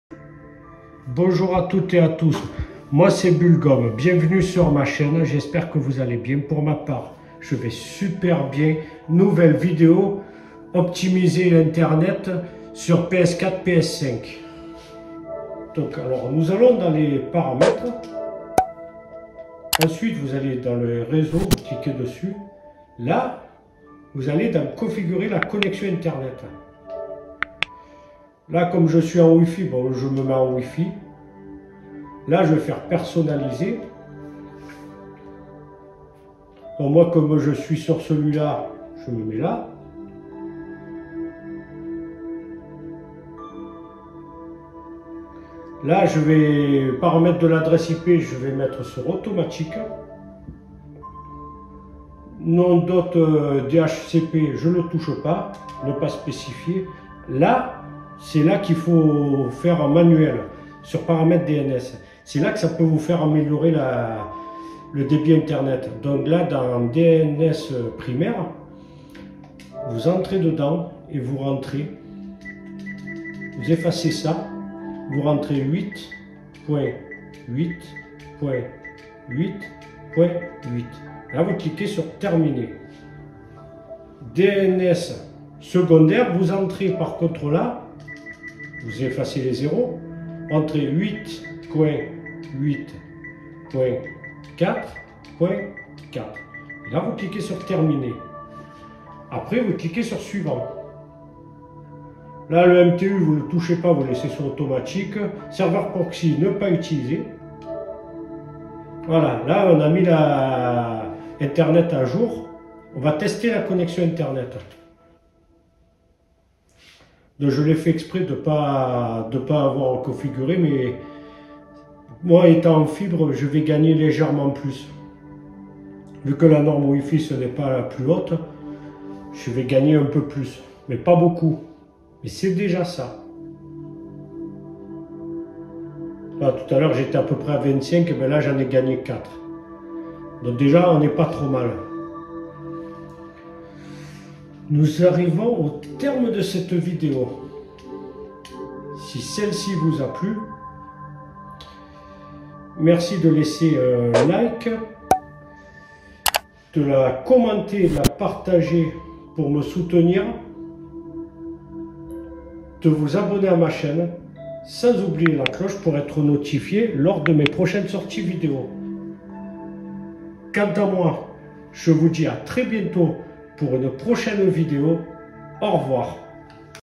Optimiser internet sur ps4 et ps5
permet de gagne plus de débit internet sur ps4 et ps5